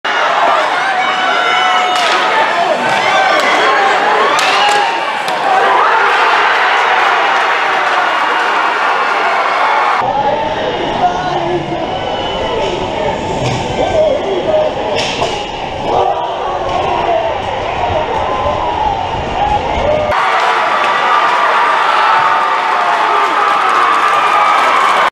2022-02-23 Ares Miller Game Winning Goal Aquinas vs Webster Schroeder
Aquinas won 6-5 in overtime.